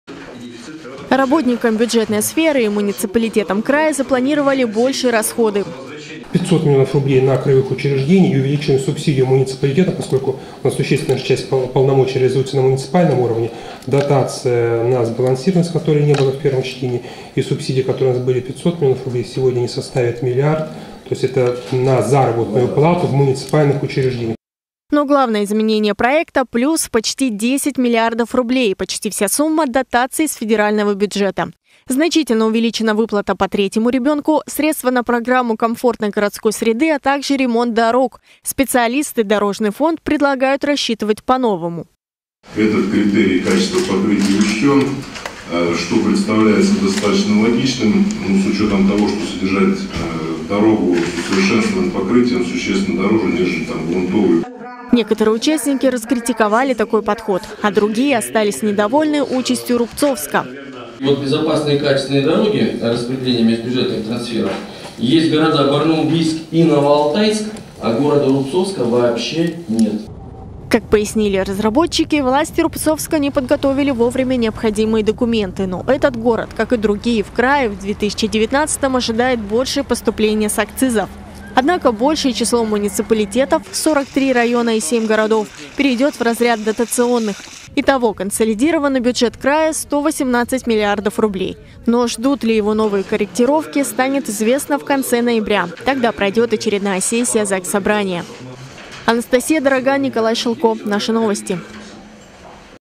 Объем краевой казны впервые превысит 100 миллиардов рублей
Краевые депутаты вернутся к обсуждению бюджета региона, который ранее был одобрен в первом чтении. Накануне парламентарии  проголосовали за его увеличение на 2019-ый год. С учетом изменений главный финансовый документ региона составит больше ста миллиардов рублей.